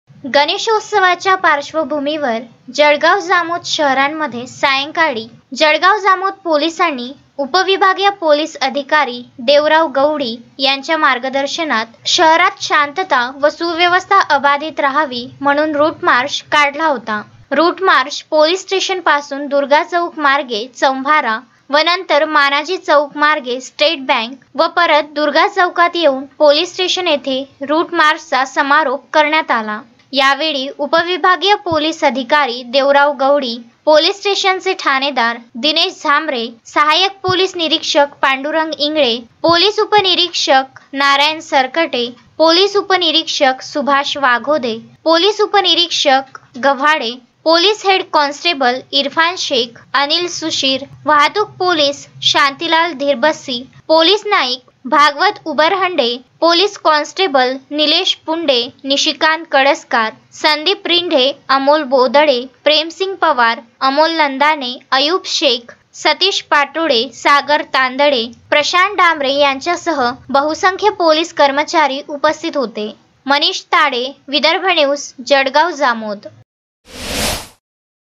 Jalgaon Jamod | गणेशोत्सवाच्या पार्श्वभूमीवर पोलिसांचा रूट मार्च
विदर्भन्यूज365मराठी गणेशोत्सवाच्या पार्श्वभूमीवर पोलिसांचा रूट मार्च

शांतता व सुव्यवस्था अबाधित राखण्याचे आवाहन